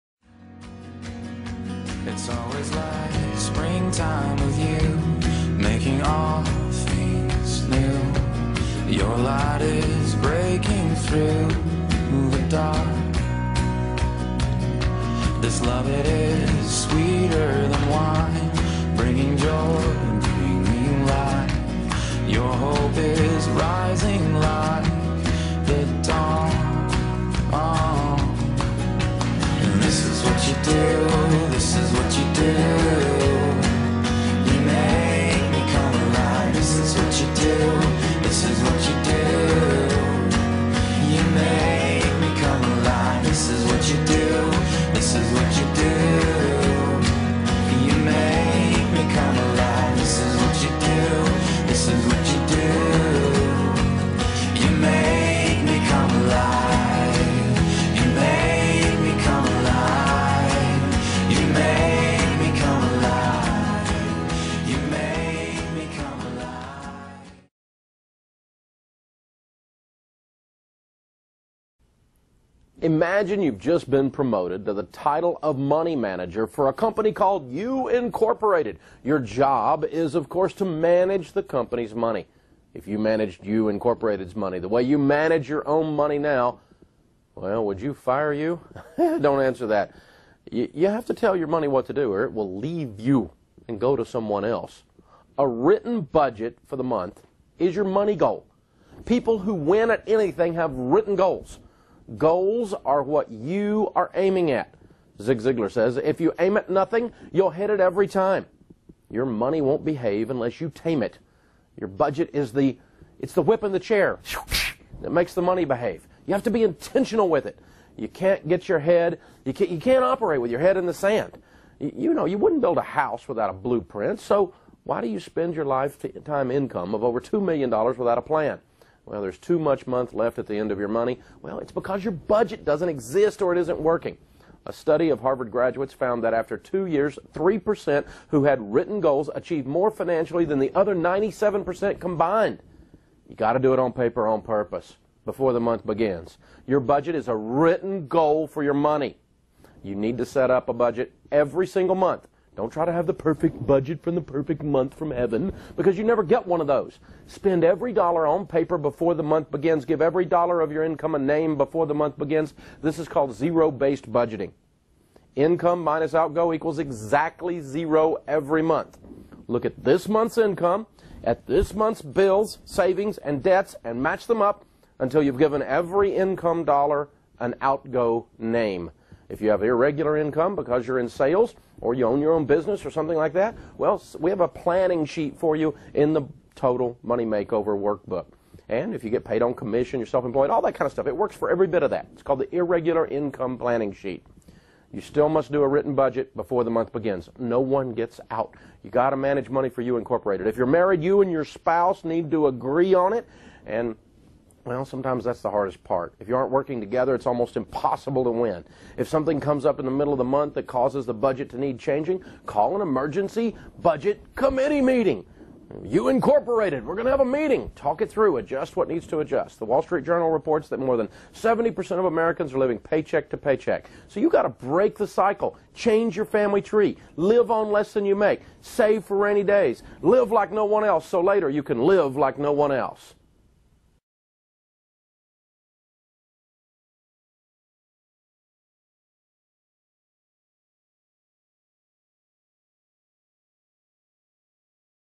Victorious Living Announcents for January 13 2013
Upcoming events for January at Victorious Living. Sunday January 13 is debt elimination Sunday! Join us as together we help defeat debt in each others lives!